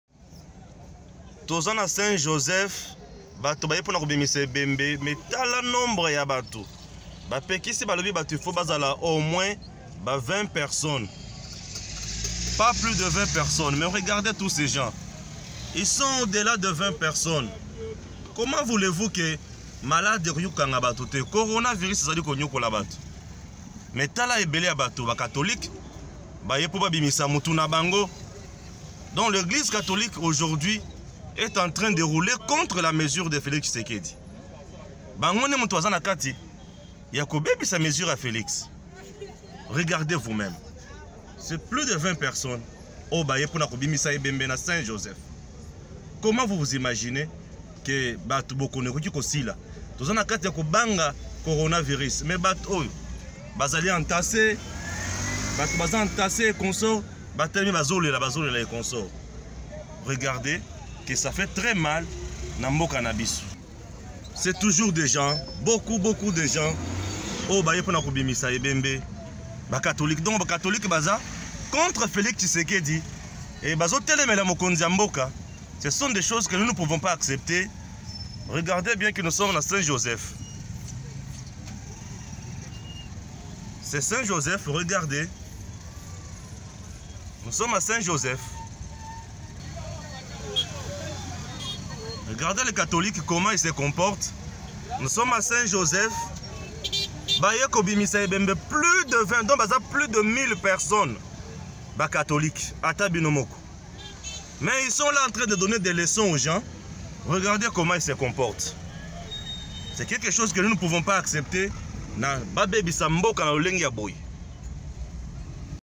EDEM SANTÉ : ÉGLISE CATHOLIQUE DEFIE ET SABOTE FELIX TSHISEKEDI, SES MESURES
L'église catholique fait bon lui semble en ce qui concerne le COVID19